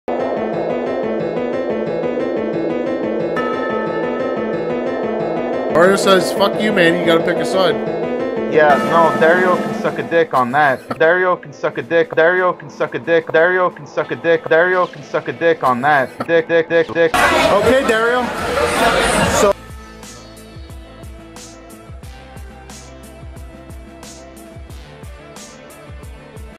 The Final Shot Podcast EP 28 Trailer MANNY VS DARIO VS DICK
Tune in this Wednesday Halloween for Episode 28 of the Final Shot Podcast